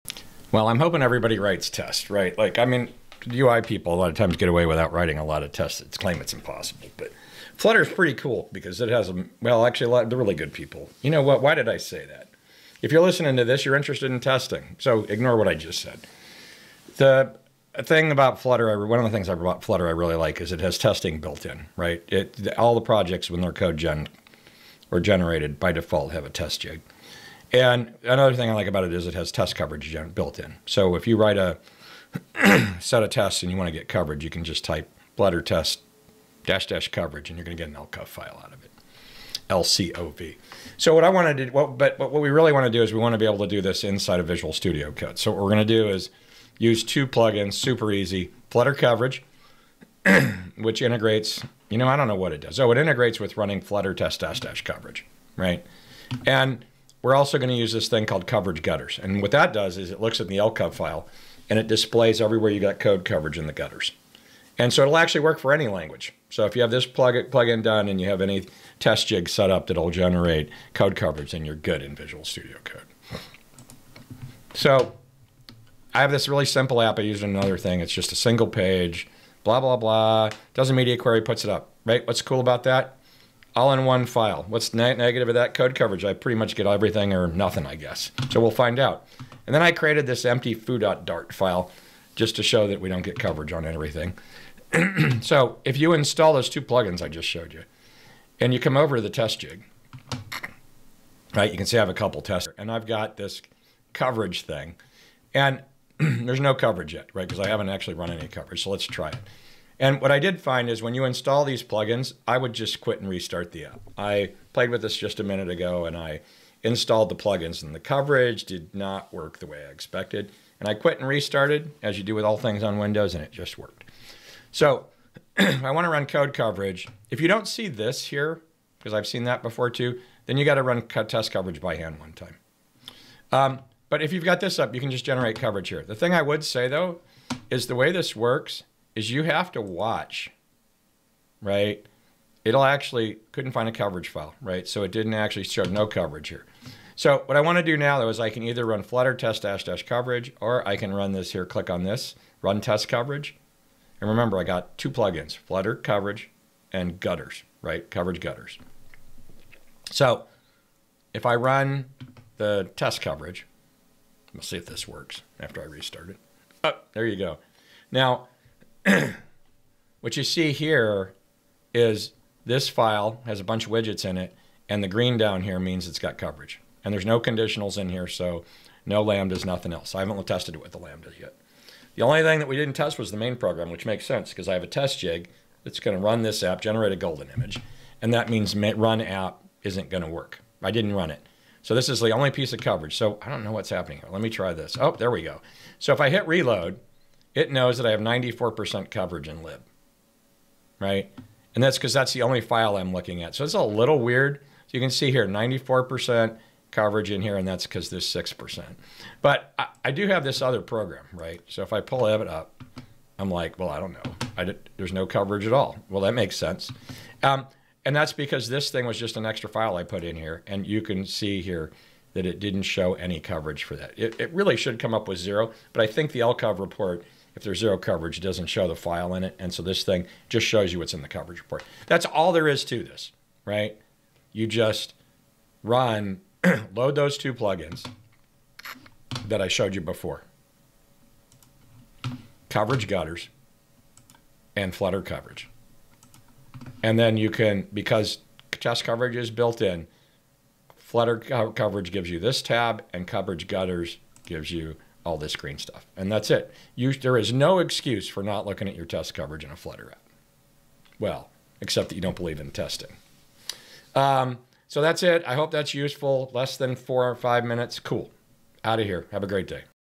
Flutter and Dart test coverage in VS Code with Flutter Coverage and Coverage Gutters
View Flutter and Dart's test coverage in VS Code two VS Code Plugins.  Leverage flutter's built-in test coverage analyzer 'flutter test --coverage'.